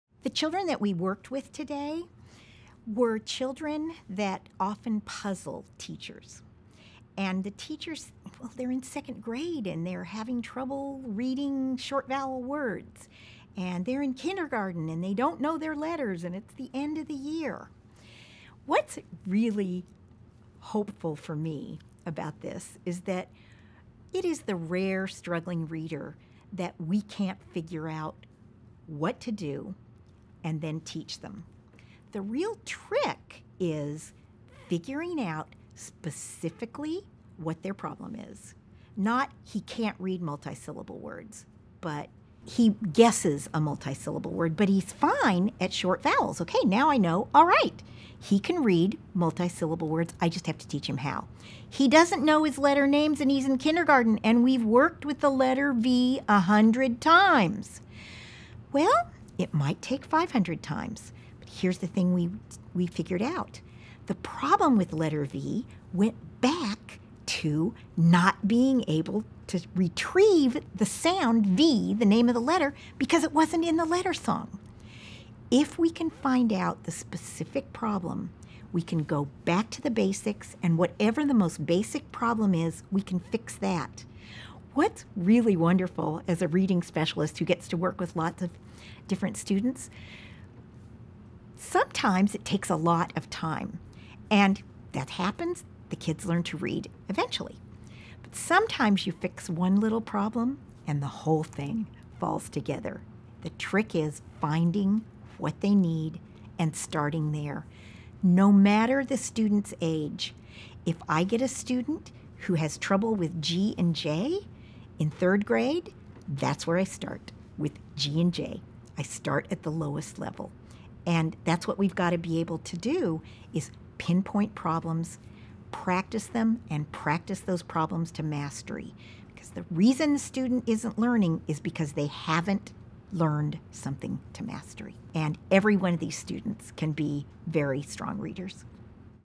Reading expert Linda Farrell: How we can help struggling readers
This interview with reading expert Linda Farrell is part of the Reading Rockets special series, Looking at Reading Interventions. Watch additional interviews with Ms. Farrell as well as one-on-one tutoring with K-3 students